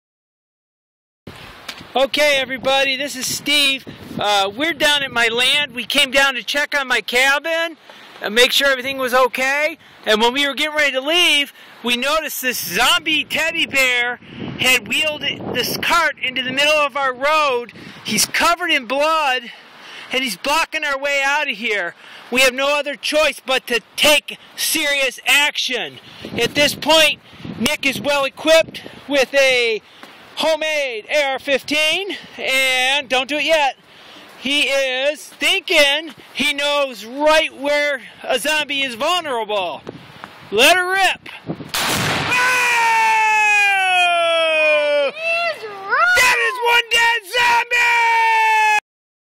Zombie Destruction - AR15 first kill!!
Rabid and Deadly Zombie Teddy Bear attempting to kill us is disabled!